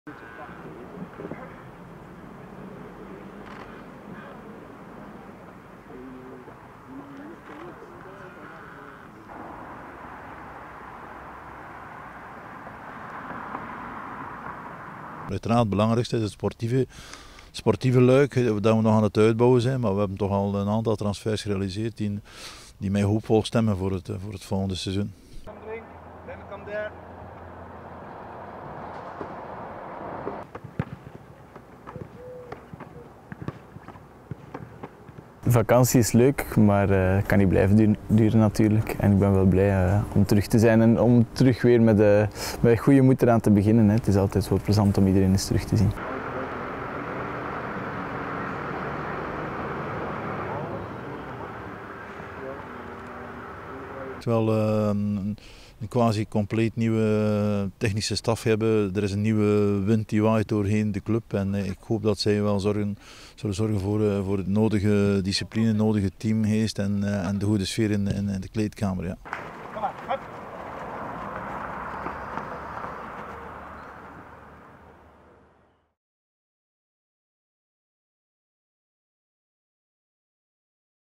KV Kortrijk- Eerste training
Deze ochtend trainde onze A-kern voor het eerst onder leiding van onze nieuwe coach Yannis Anastasiou en de vernieuwde technische staf.

Ook onze nieuwkomers Bennard Kumordzi, Christophe Lepoint, Bryan Verboom en Sebastien Bruzzese trainden mee.

Vrijdag spelen de kerels de eerste oefenenwedstrijd tegen Wikings Kortrijk. De partij wordt gespeeld op het terrein aan de Kouter (Moorseelsestraat 111b, Heule). Aftrap om 19u.